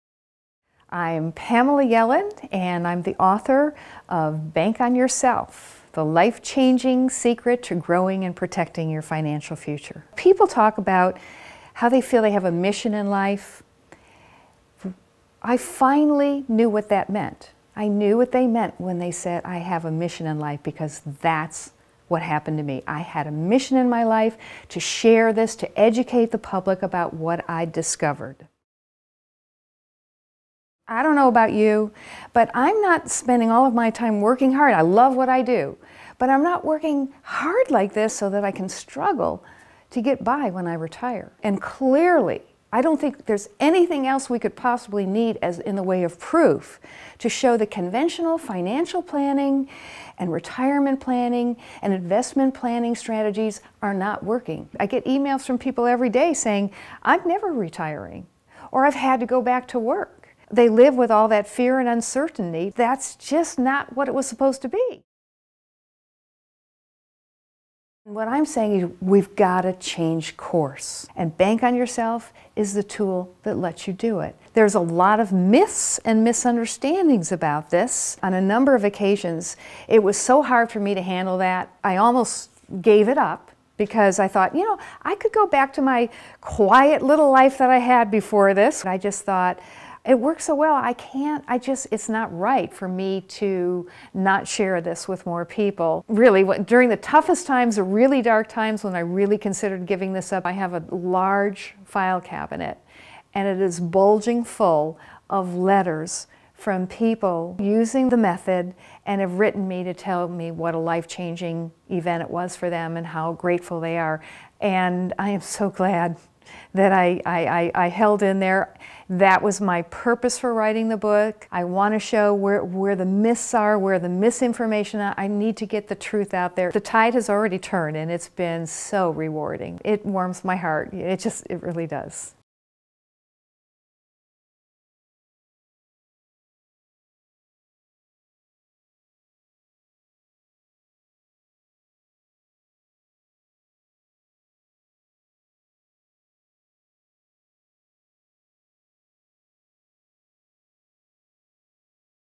Financial Advice about Retirement Planning from Pamela Yellen
Frustrated with her experience with conventional retirement planning and investment strategies, author and founder of Bank On Yourself, Pamela Yellen, found her mission in life - sharing a proven method of financial planning that gives new hope to those desiring a better way of planning for the future.

Many people are struggling with their finances. The Bank On Yourself method helps break away from conventional investing for a more secure, stable future. There are a lot of myths and misunderstandings about Bank On Yourself, but for the people Pamela has helped, Bank On Yourself has been a life-changer.